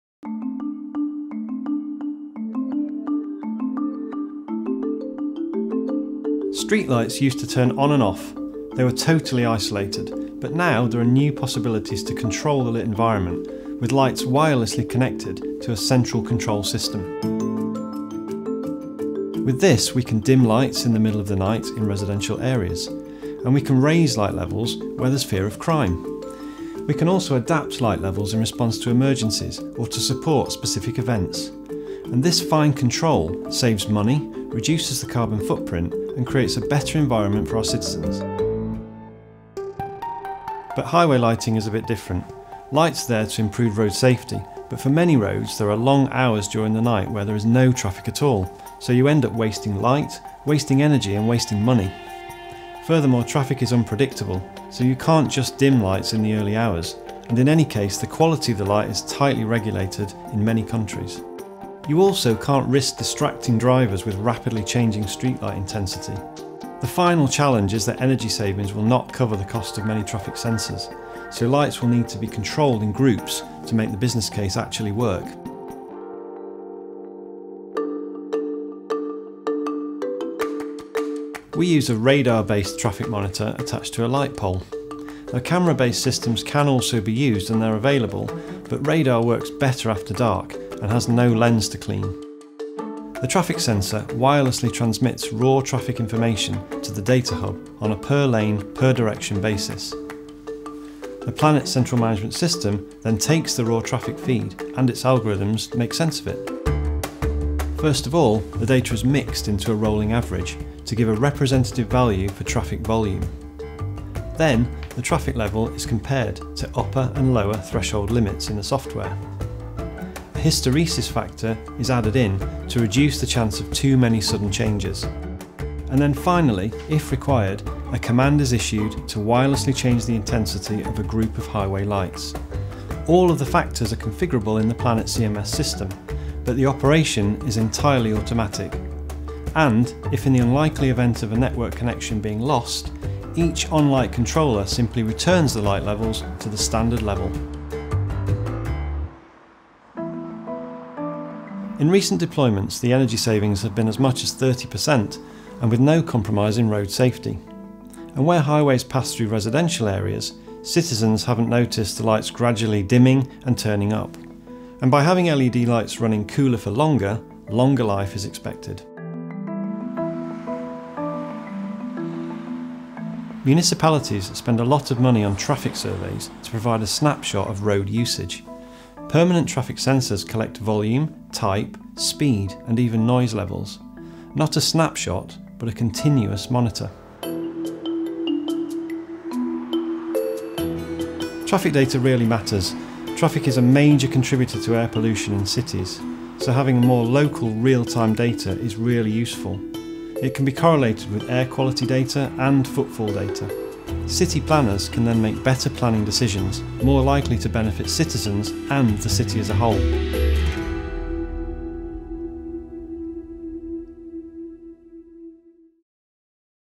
Traffic adaptive street lighting
Lighting empty highways is wasteful but failing to light a busy highway can be dangerous. Traffic adaptive lighting helps reducing energy costs on areas where timed programs aren't safe to use.